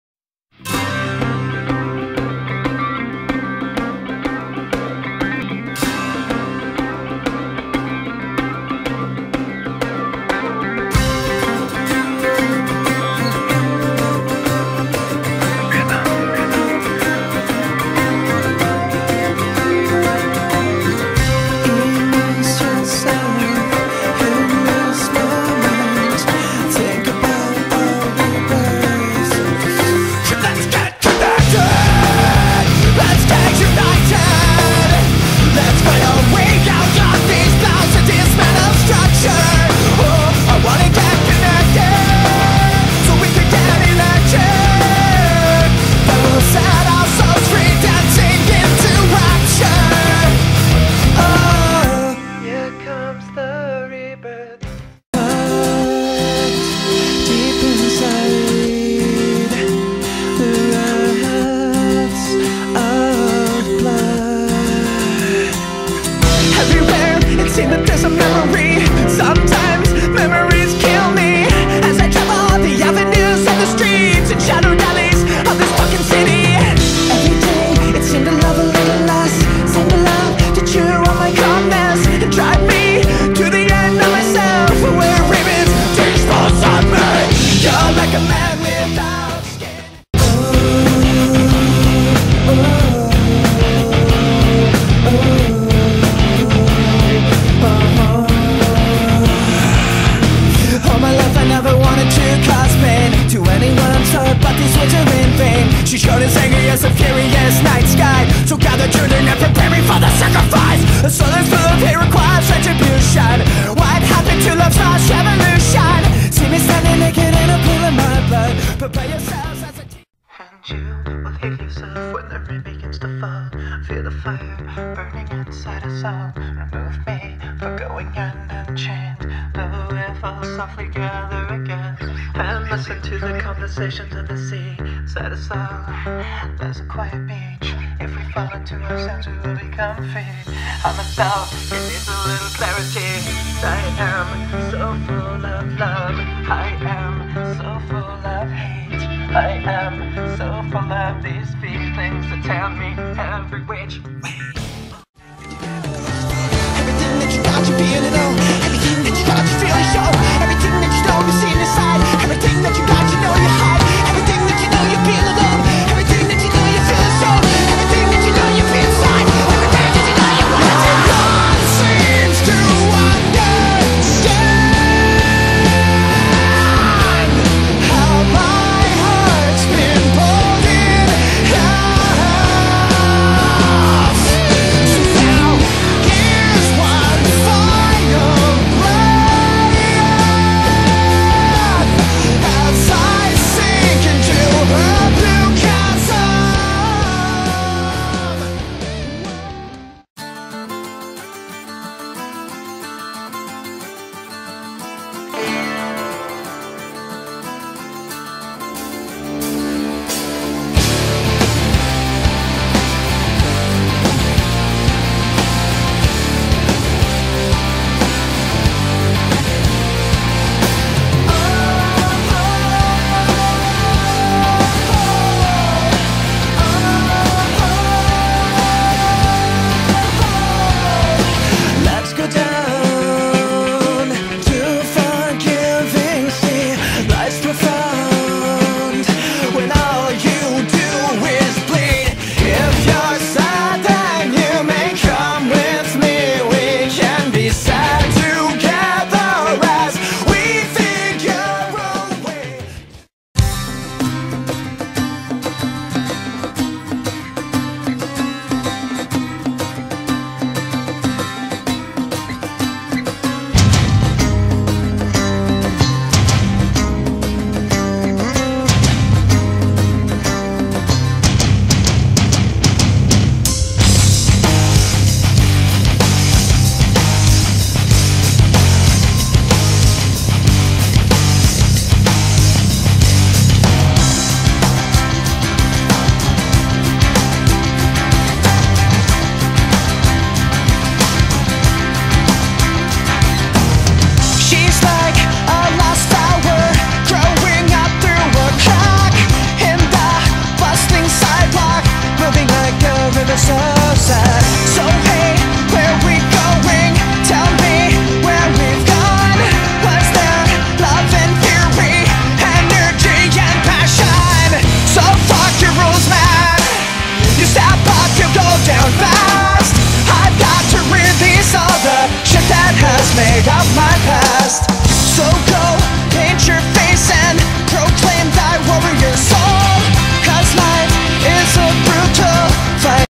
Boy Hits Car (Sampler Pack)
A collection of samples from various Boy Hits Car songs to help spread the word about the band and Lovecore.

New Music From their album due in early 2011